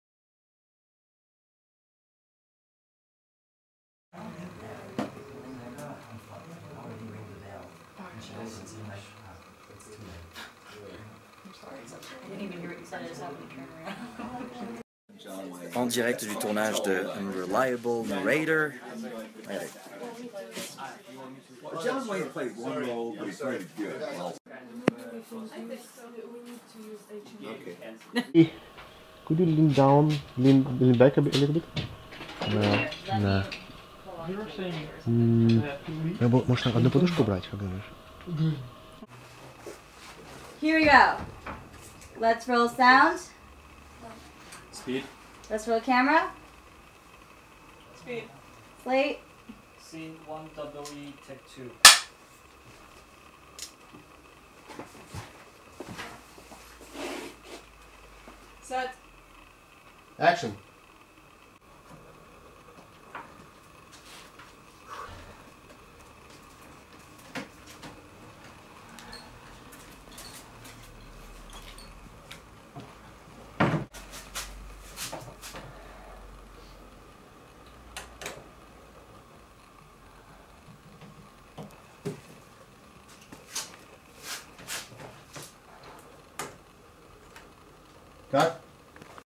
Unreliable Narrator - Behind the scenes (2016)
Sneak peek from the shooting of the short Film "Unreliable Narrator" directed by Ruslan Ranjabar
Filmed by Alexander Naumovich
Cast: Maxence Cazorla, Jon Allen Russo